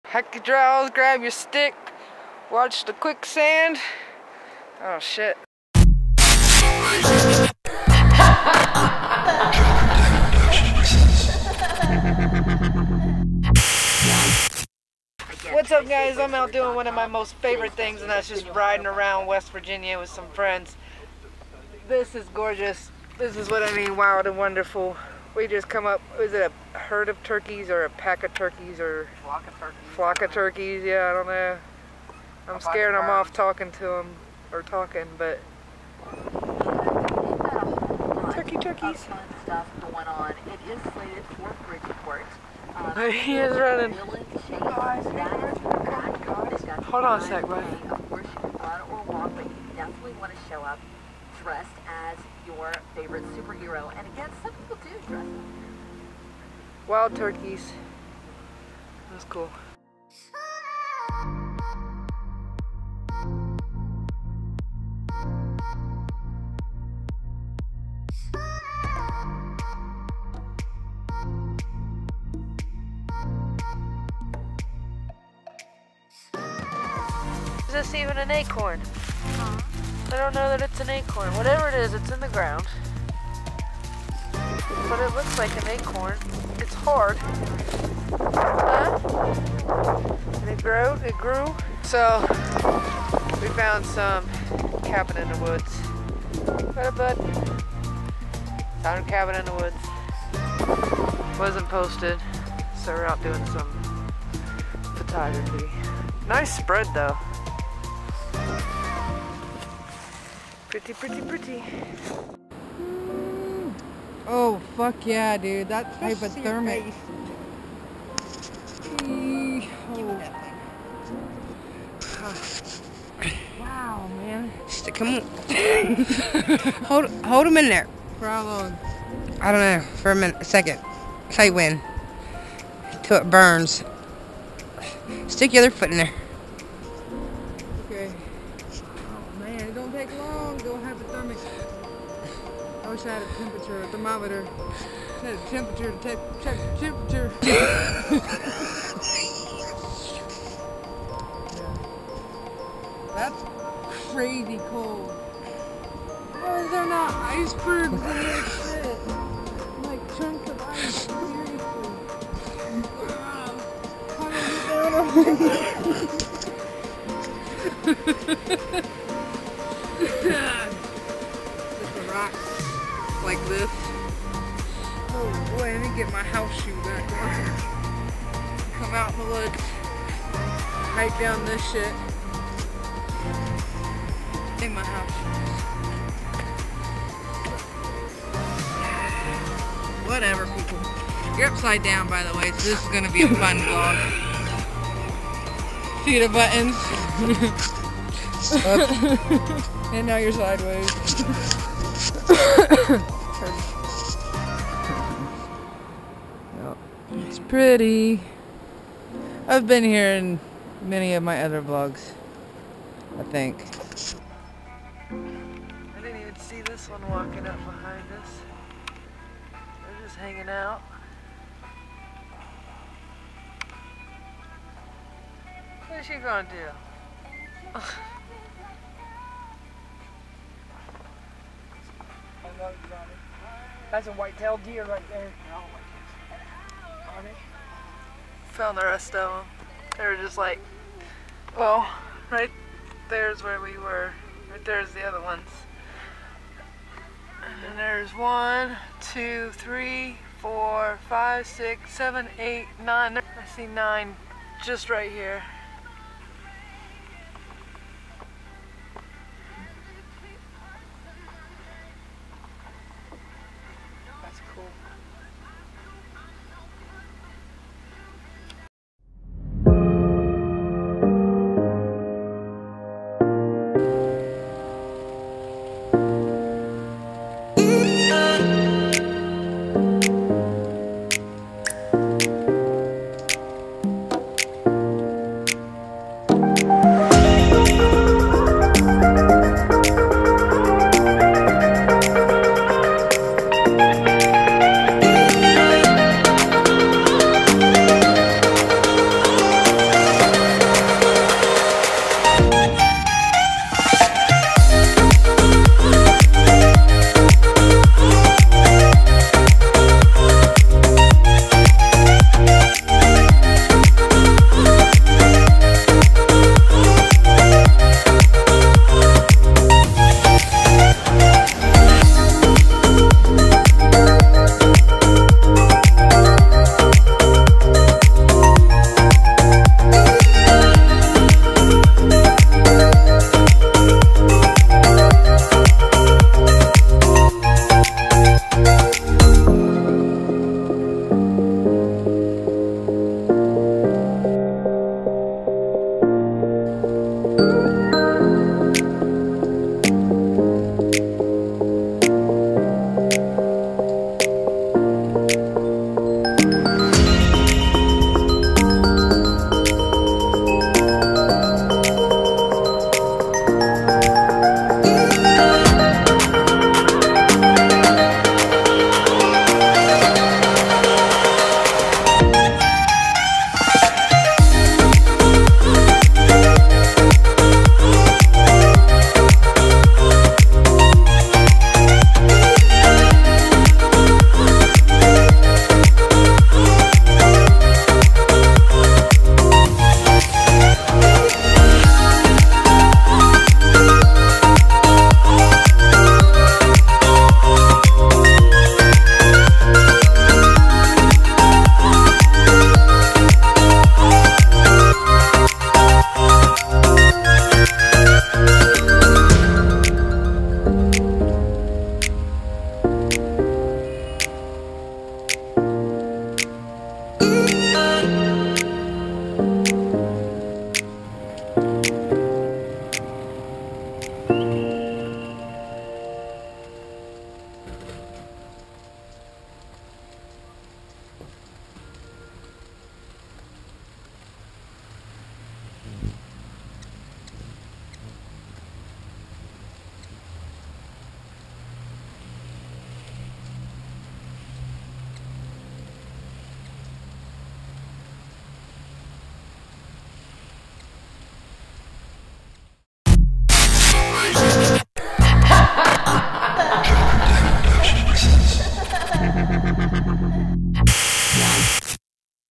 Wildlife in Wv
driven around wv saw alot of deer and wild turkey and at the end is a hummingbird montage that was shot this past fall, quick